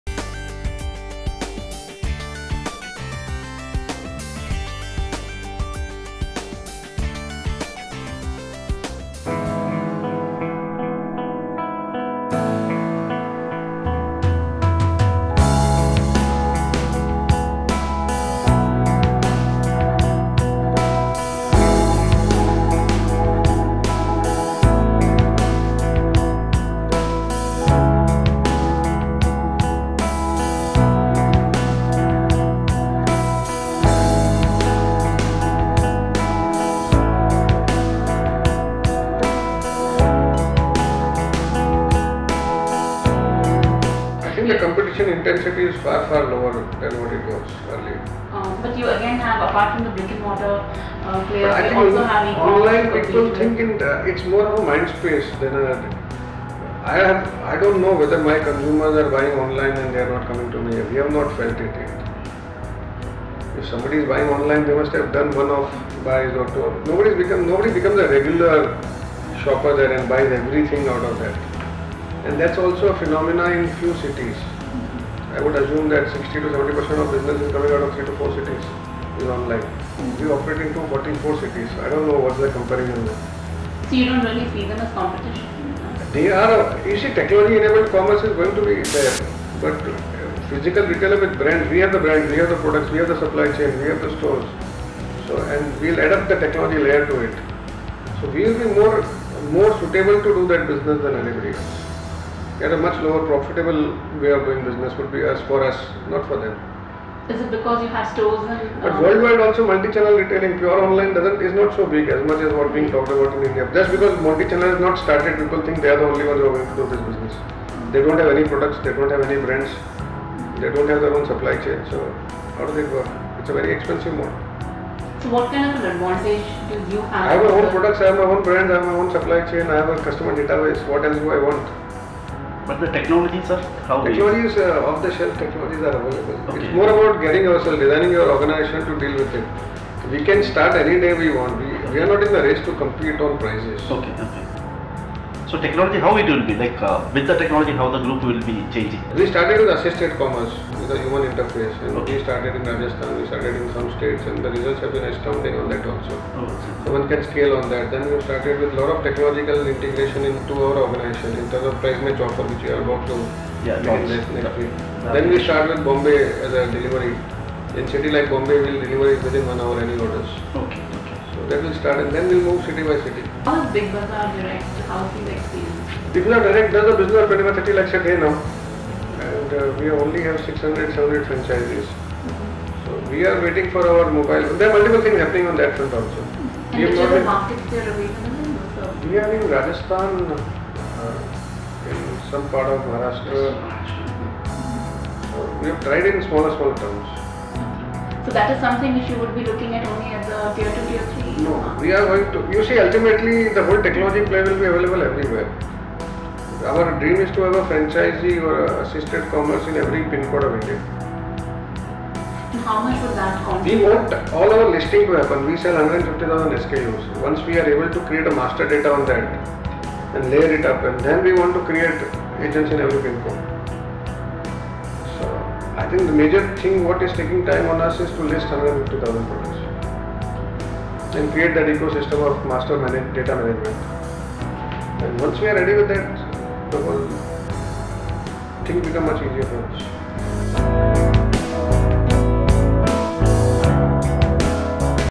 Nobody becomes a regular and buys everything online: Kishore Biyani
Kishore Biyani, Group CEO, Future Group, is unfazed by competition from online retailers. He speaks to Business Today on his own technology plans.